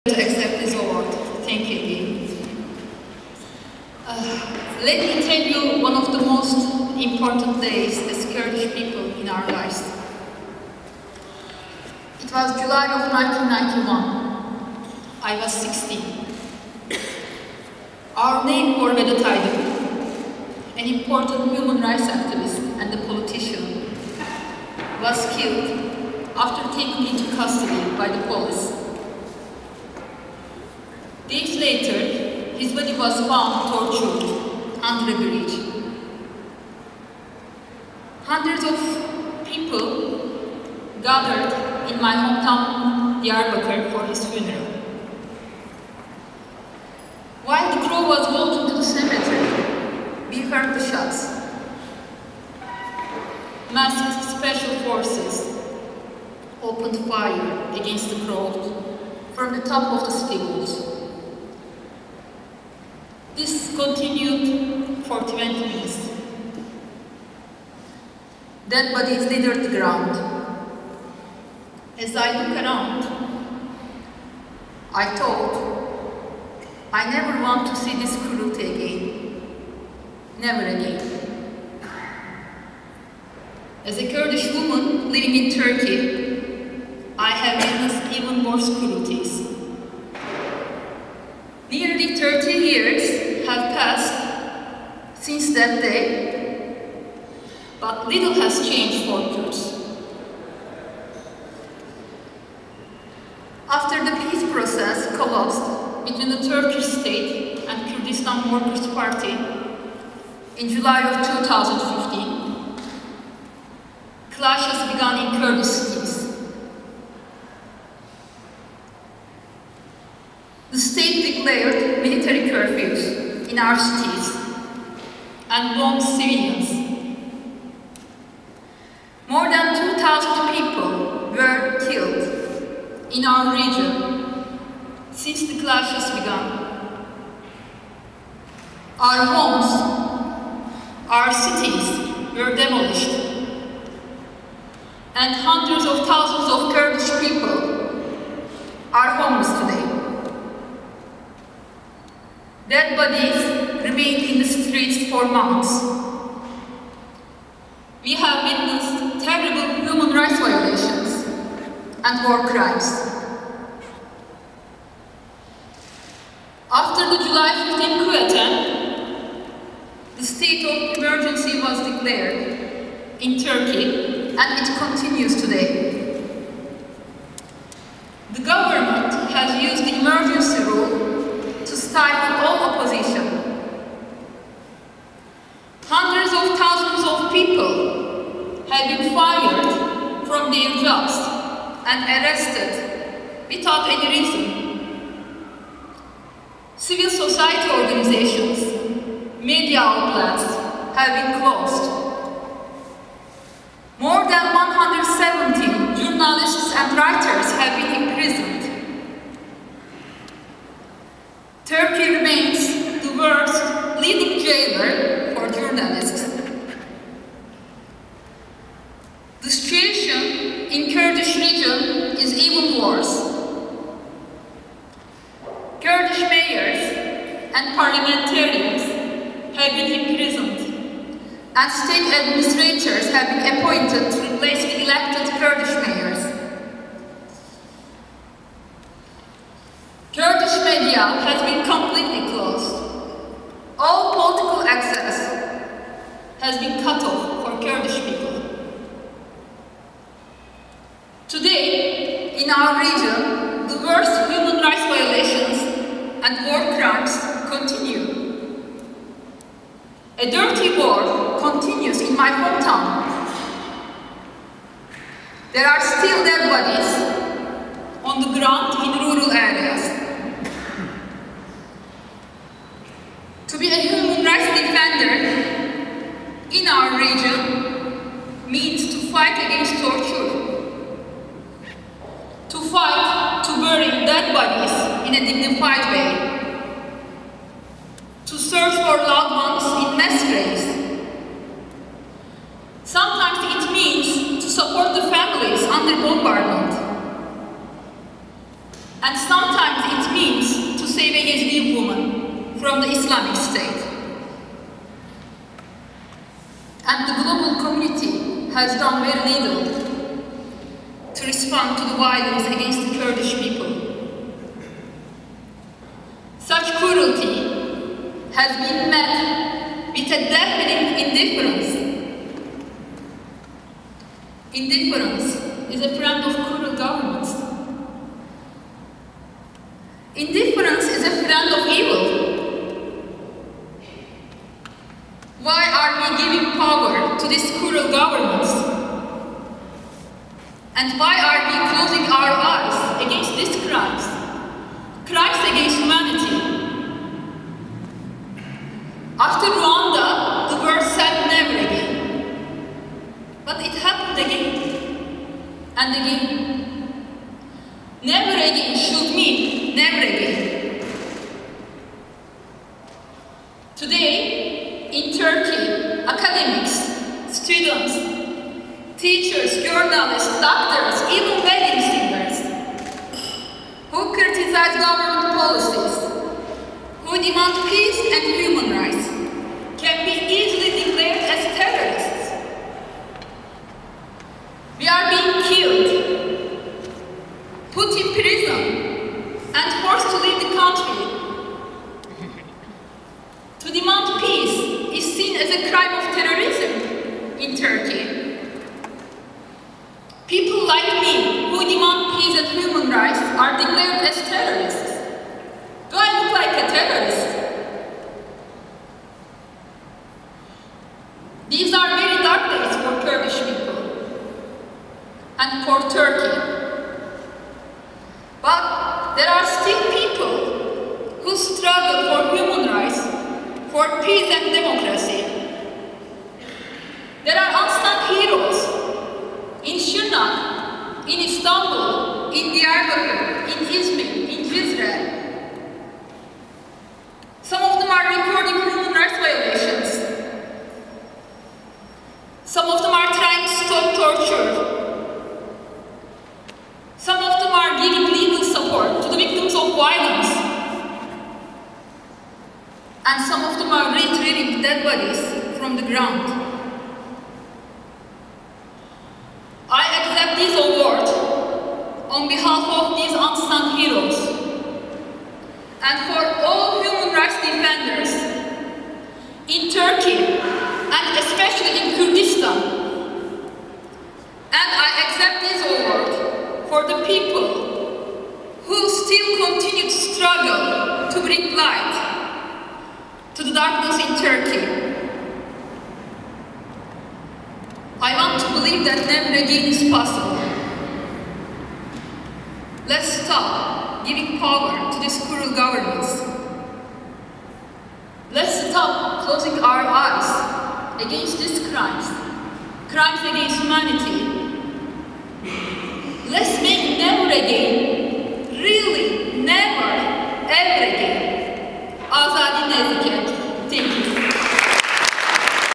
Nurcan Baysal'ın 'FLD Uluslararası İnsan Hakları Ödülü’ konuşması
Front Line Defenders- Risk Altındaki İnsan Hakları Ödülü’nü alan Nurcan Baysal, teşekkür konuşması yaptı. Baysal, konuşmasında ‘’Türkiye’de yaşayan Kürt bir kadın olarak, çok kötü zulümlere tanıklık ettim’’ diyerek şunları söyledi:

Burada olmak büyük bir onur ve bu ödülü kabul etmek büyük bir mutluluk.

Size, Kürt halkı olarak hayatımızdaki en önemli günlerden birinden bahsetmeme izin verin.

1991 yılının Temmuz ayıydı.

16 yaşındaydım.

Komşumuz, önemli bir insan hakları aktivisiti ve politikacı olan Vedat Aydın polis tarafından gözaltına alındıktan sonra öldürüldü. Günler sonra, cesedi işkence yapılmış halde bir köprünün altında bulundu. Yüz binlerce insan cenazesi için Diyarbakır’da toplandı. Kalabalık mezarlığa doğru yürürken, silah sesleri duyduk. Maskeli özel kuvvetler, şehrin surlarının tepesinden kalabalığa ateş açtı. Bu, 20 dakika kadar devam etti.  Ölü bedenler yere dağılmıştı.

O gün alana baktığımda, “Bu zulmü bir daha asla, asla görmek istemediğimi” düşündüm.